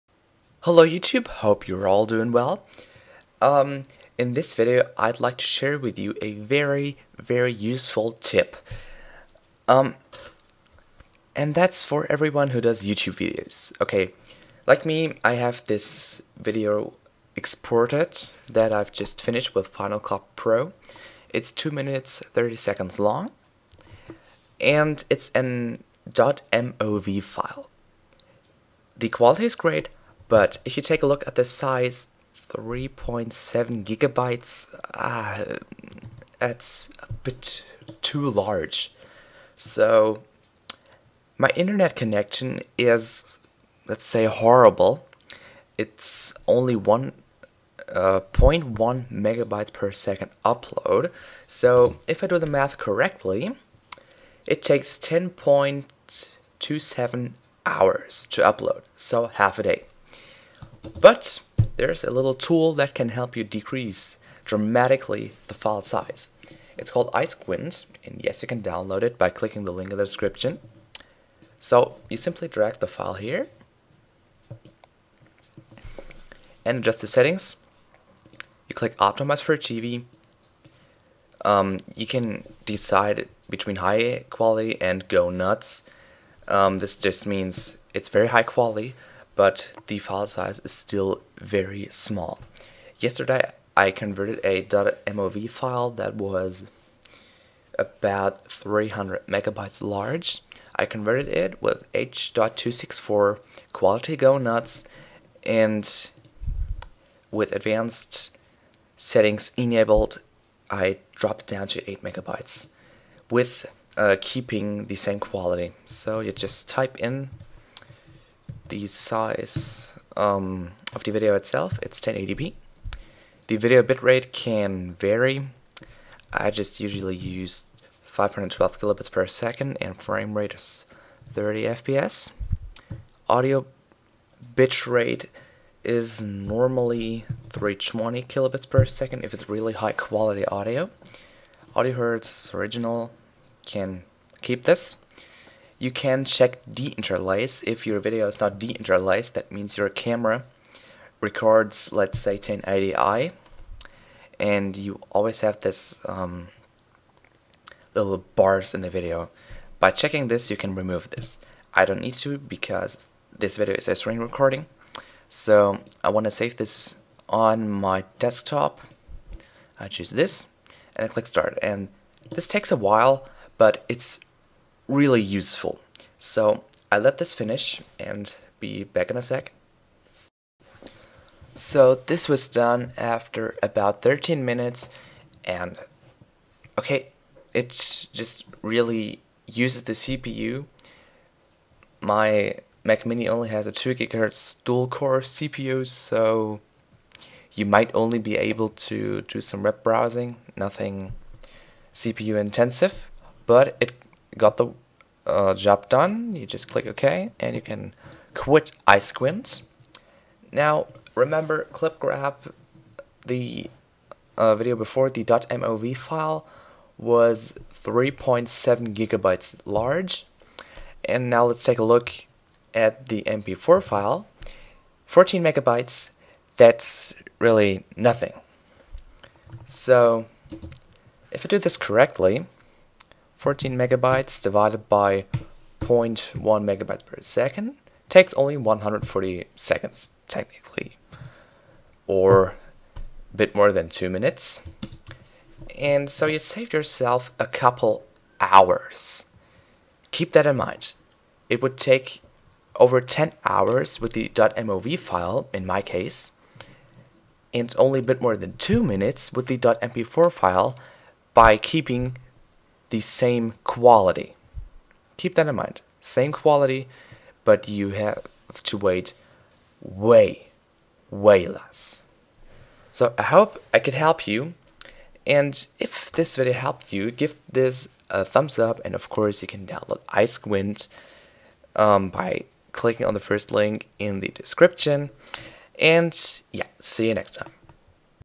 How to decrease video file sizes without degrading quality
Hey YouTube! This video shows you how you can convert video files from .mov into .mp4. The file size decreases dramatically by maintaining the quality.